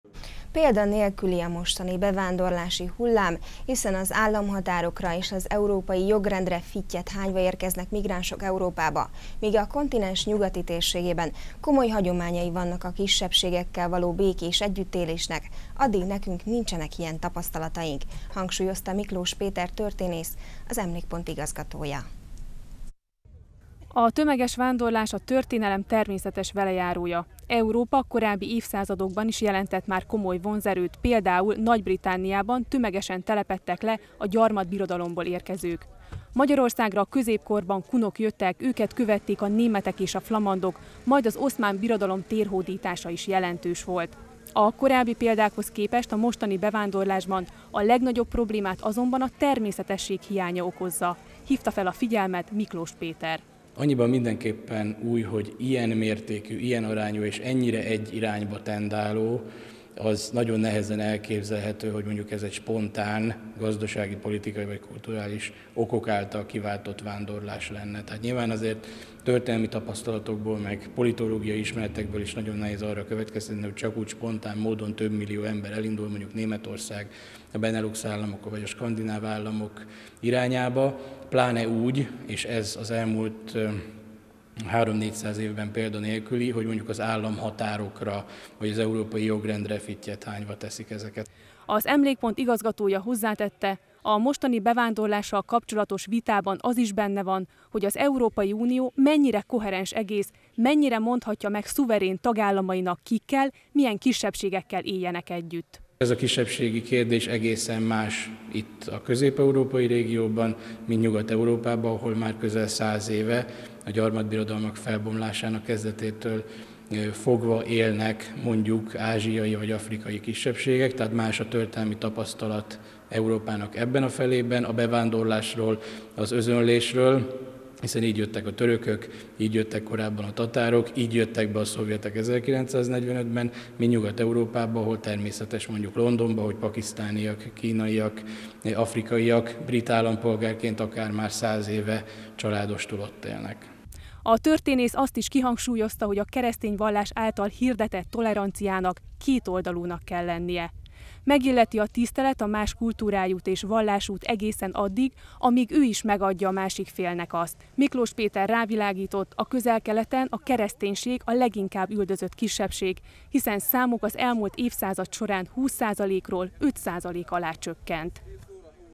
Miklós Péter: példanélküli a mostani bevándorlási hullám
Példanélküli a mostani bevándorlási hullám, hiszen államhatárokra és az európai jogrendre fittyet hányva érkeznek migránsok Európába. Míg a kontinens nyugati térségében komoly hagyományai vannak a kisebbségekkel való békés együttélésnek, addig nekünk nincsenek ilyen tapasztalataink - hangsúlyozta Miklós Péter történész, az Emlékpont igazgatója.